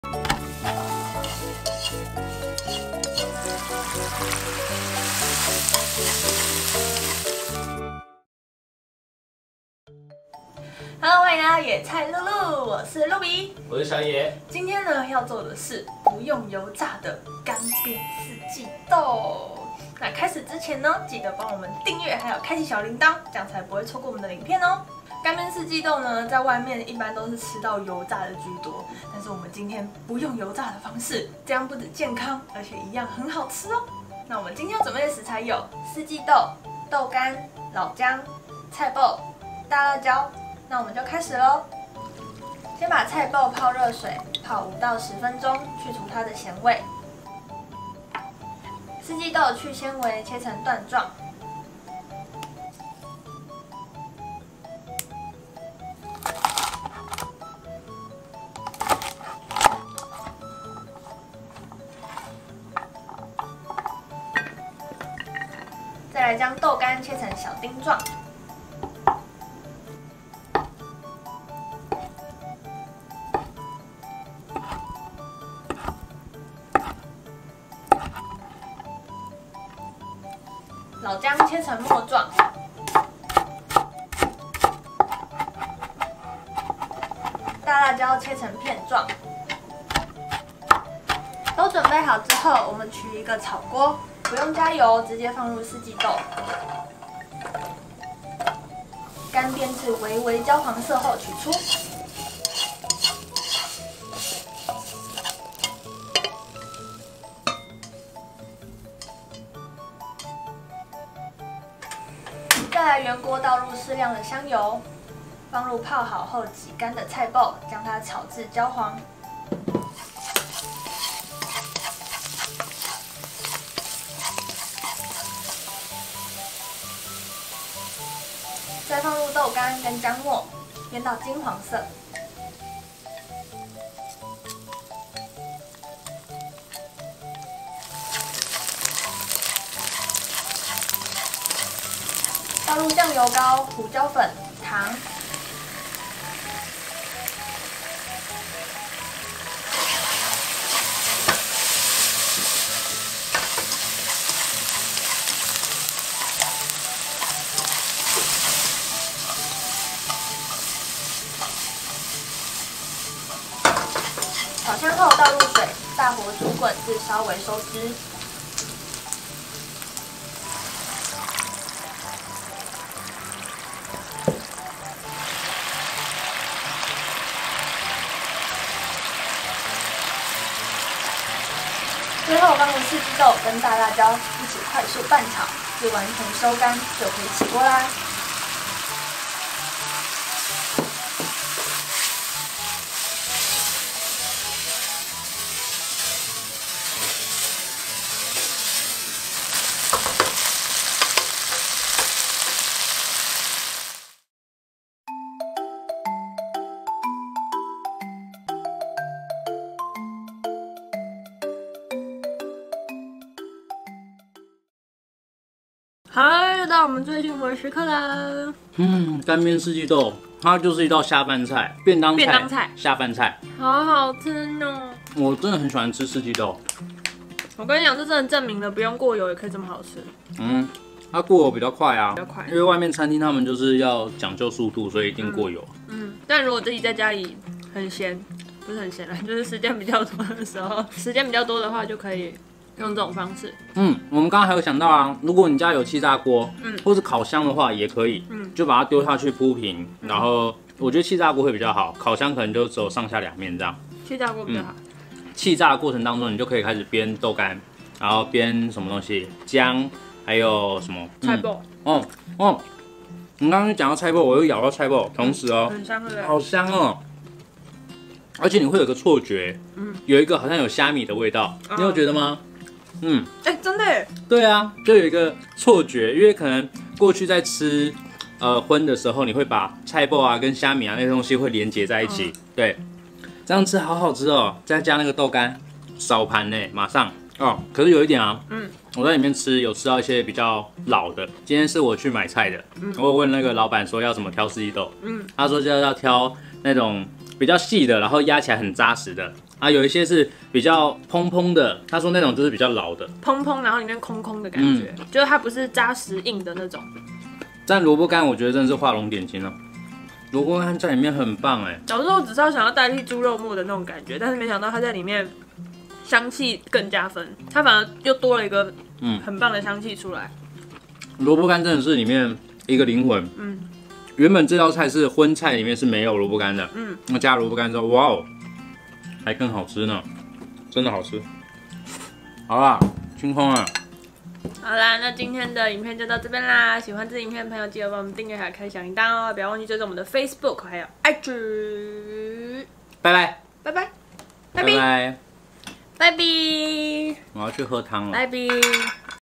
素食乾煸四季豆：熱炒店必點🔥加了這個居然還有蝦米味⁉️而且免油炸營養又健康｜素食 純素 全素｜素食料理超簡單｜➤野菜鹿鹿 Veggie Deer
以往葷食的乾煸四季豆都會有蝦米在裡面
但變成素食之後
加了這樣食材
居然也會有蝦米的味道
真的太神奇了！
而且不用油炸的方式
也能做出熱炒店的味道
不僅更健康
也能讓你在家裡方便做出熱炒味！
趕快動手做做看吧！

食材：
四季豆 350g
豆乾 4塊
老薑 15g
菜圃 30g
大辣椒 1條

調味料:
醬油膏 30g(2 tbsp)
胡椒粉 3g(1tsp)
糖         8g(1 tbsp)